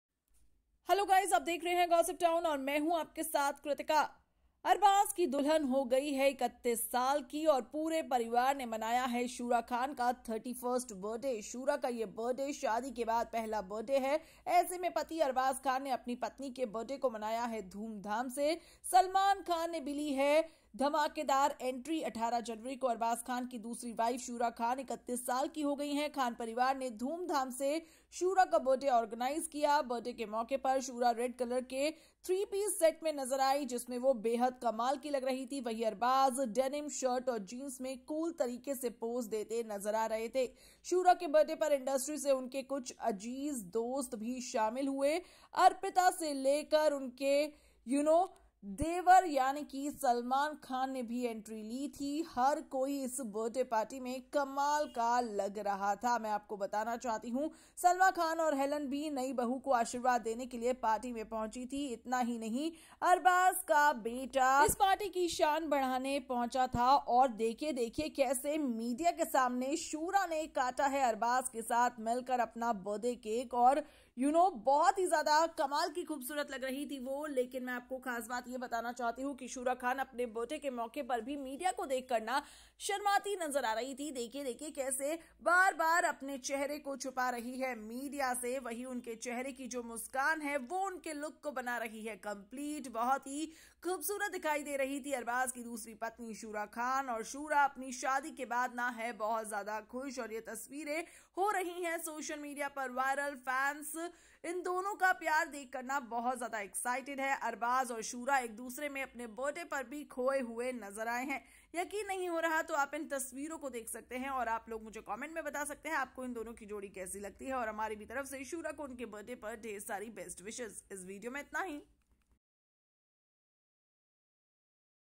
Arbaaz Khan की पत्नी Shura Khan birthday पर दिखी ऐसे, Shura Khan looks beautiful on birthday
arbaaz khan second wife Shura looks beautiful on birthday party. Watch the video to know more about this.